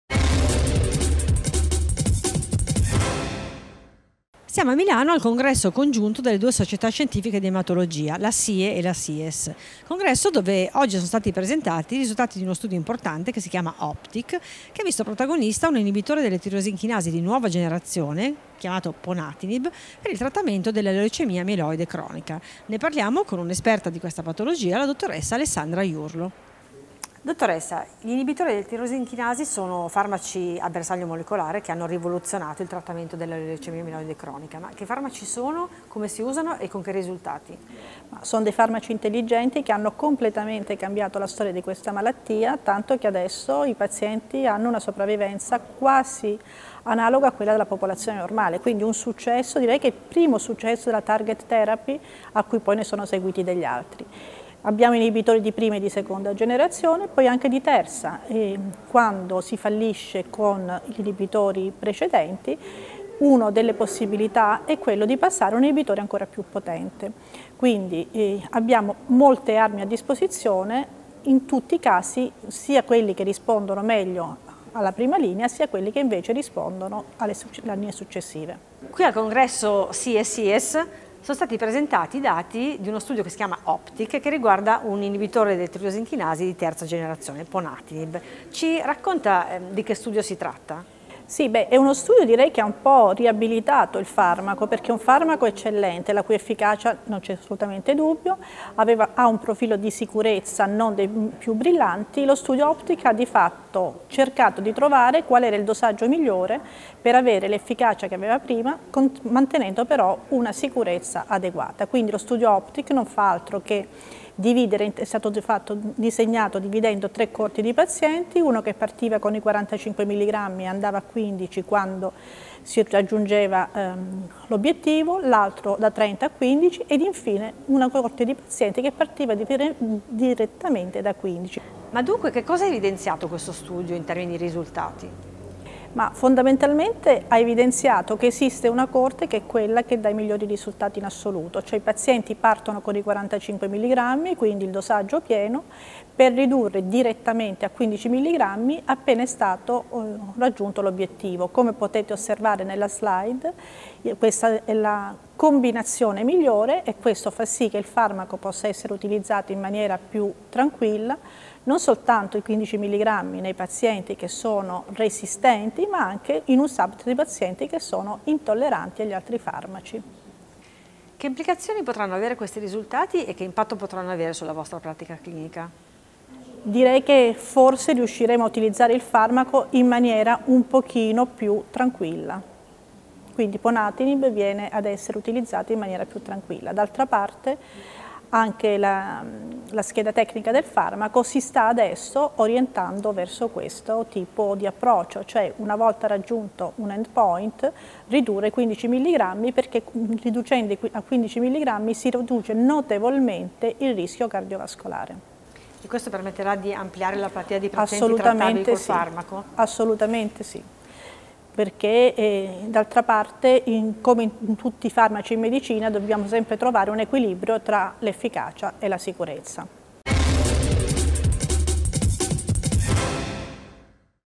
Leucemia mieloide cronica, possibile ridurre il dosaggio di ponatinib senza perdere in efficacia
In quest’intervista alla Dottoressa Alessandra Iurlo, Responsabile dell’UOS Sindromi Mieloproliferative del Policlinico di Milano, parliamo di leucemia mieloide cronica e del risultati dello studio OPTIC, presentati a Milano durante il congresso congiunto della Società Italiana di Ematologia (SIE) e della Società Italiana di Ematologia Sperimentale (SIES). I dati di questo studio, focalizzati sul trattamento con l’inibitore delle tirosin chinasi (TKI) di terza generazione ponatinib, evidenziano che si può ridurre il dosaggio per prevenire gli eventi avversi senza perdere in efficacia, rendendo possibile un impiego del farmaco anche nei pazienti intolleranti